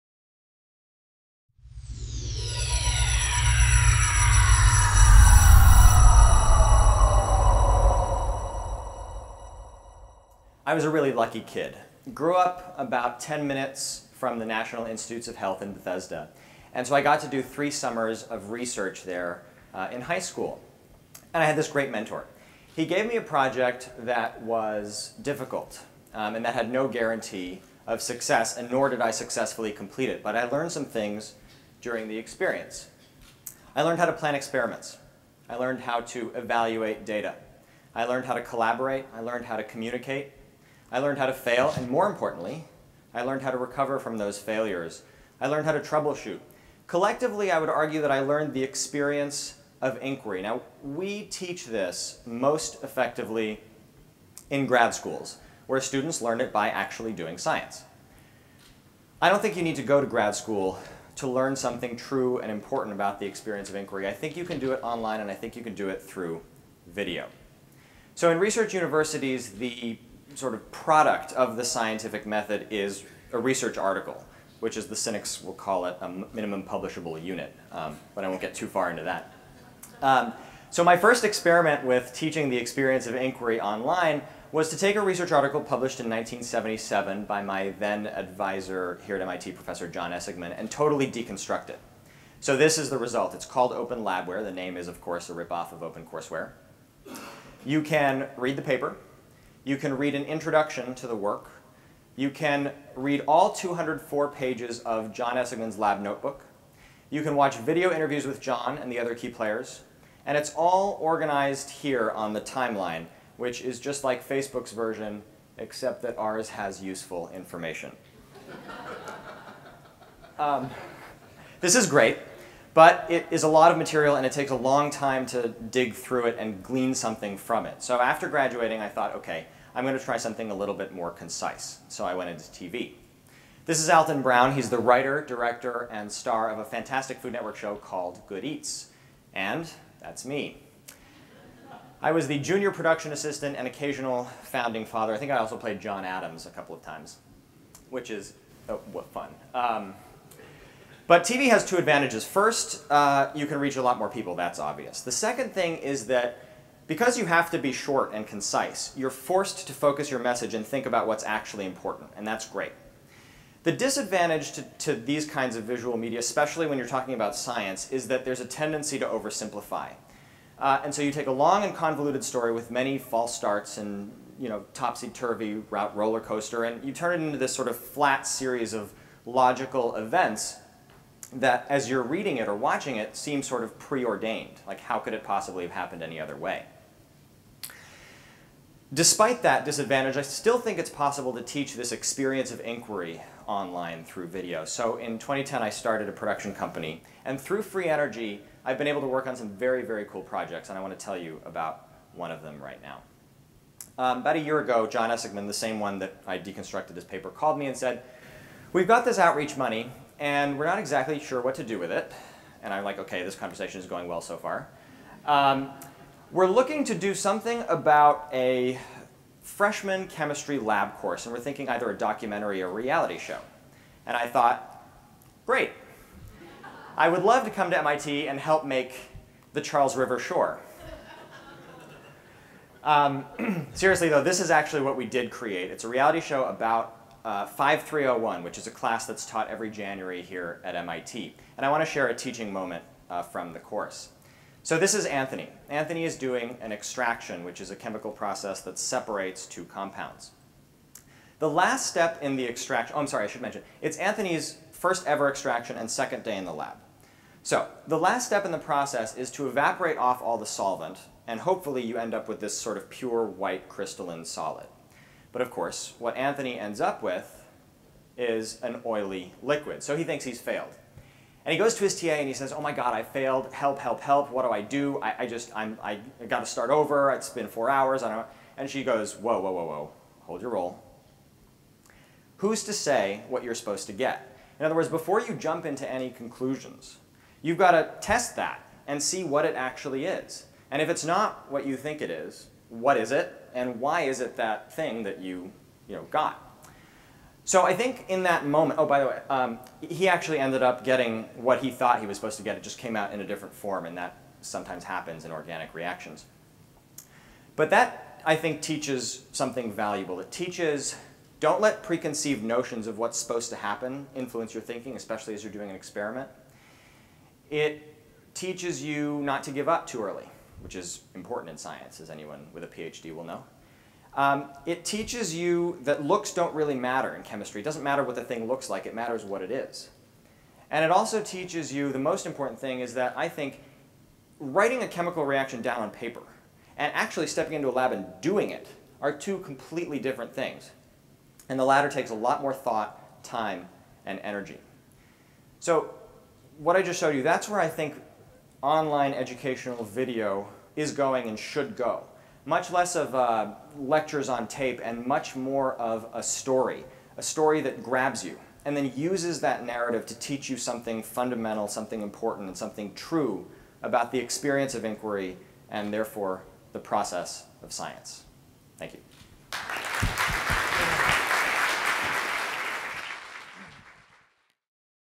Emerging Leaders Forum : George Zaidan
George Zaidan is an MIT '08 graduate and a Goldwater Scholar who has written and hosted for The Weather Channel and worked on Alton Brown's science/cooking Opus, Good Eats. He founded Free Energy Productions LLC. He has produced video for MIT, IBR, Sangari Active Science, Discover Magazine and New Scientist; and created a web series called Pocket Science that performs lossless compression on research articles to form 3-minute bites.